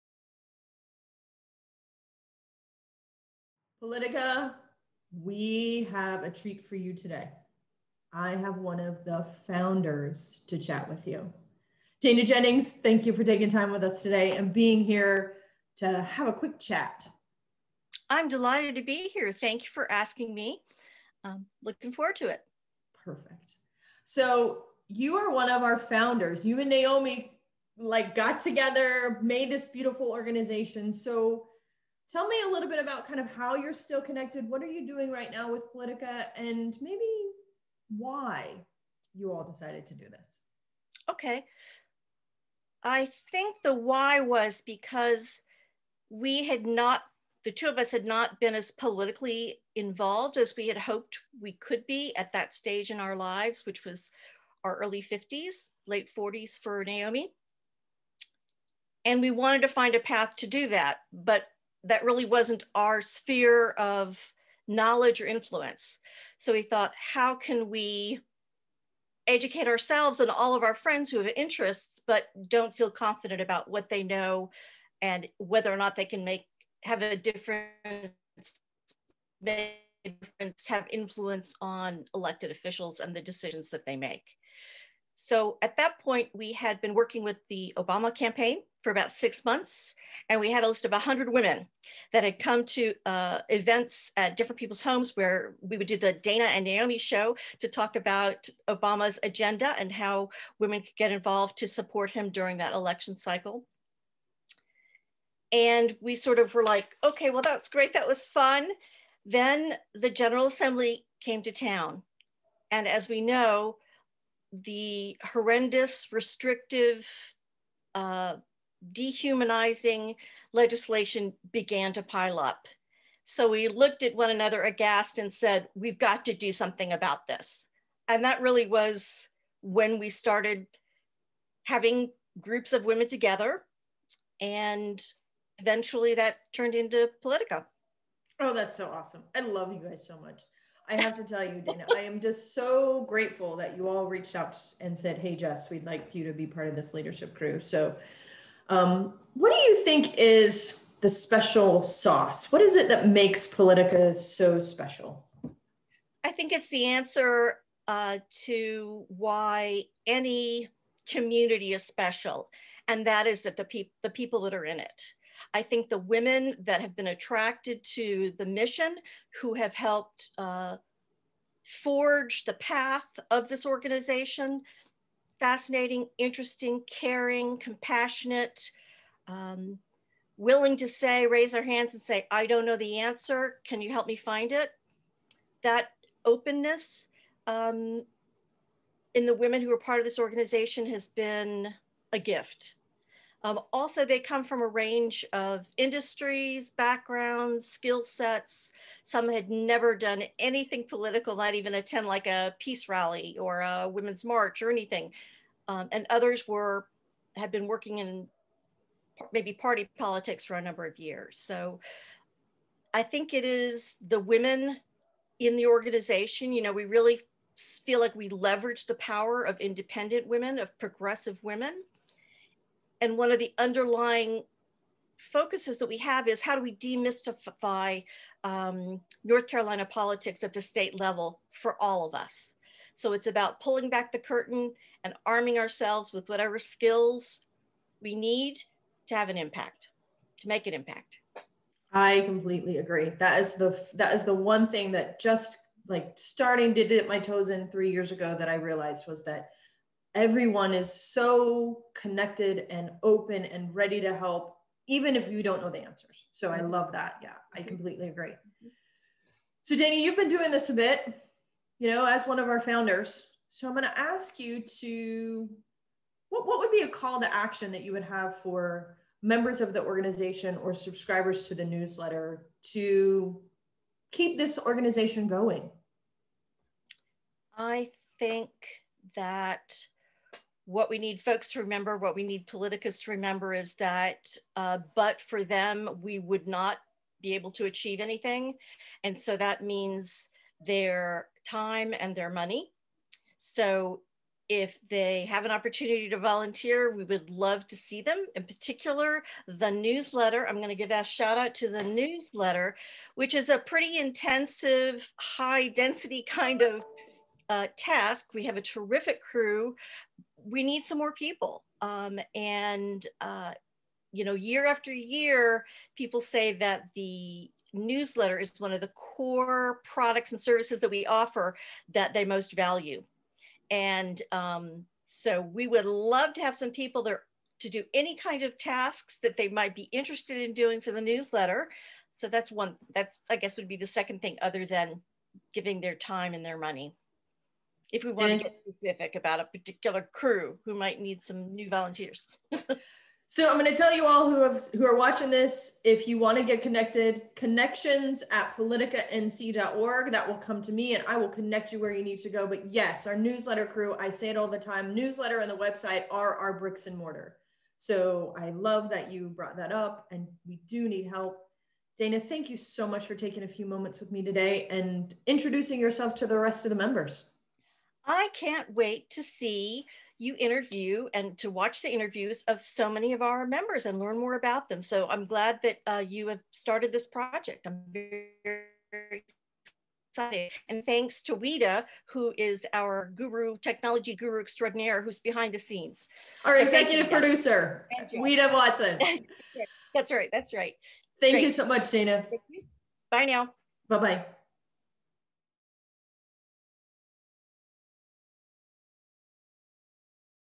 PoliticaNC Events Crew
Meet Dana Jennings, Co-Founder of PoliticaNC and Co-Leader of the Events Crew. Learn more about how PoliticaNC came about and what she thinks differentiates PoliticaNC from other activist groups.